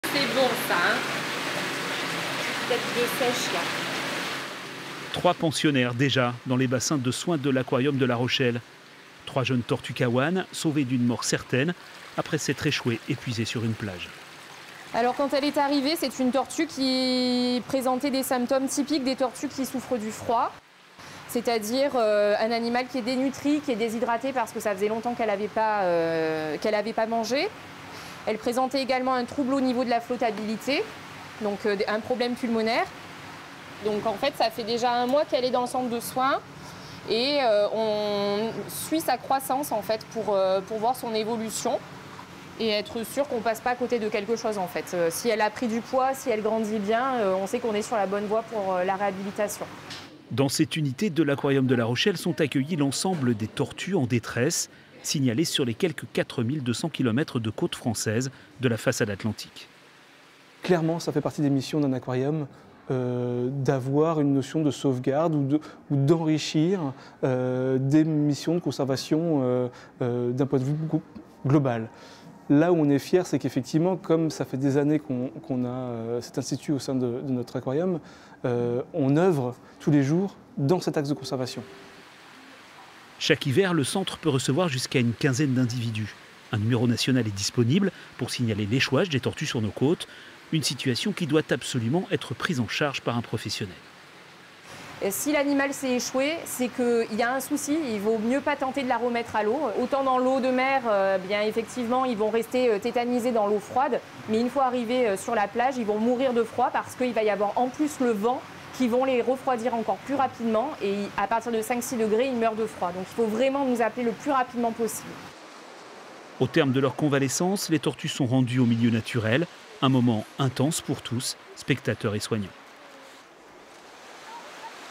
Nouvelles tortues à l'aquarium de La Rochelle
ON va terminer ce journal avec la belle histoire de ces 3 tortues de mer... Retrouvées échouées sur la plage, elles se refont actuellement une santé dans les bassins de l'aquarium de la Rochelle. Si tout va bien, elles retrouveront prochainement le large.
Reportage de Sébastien Poirier et Marc Millet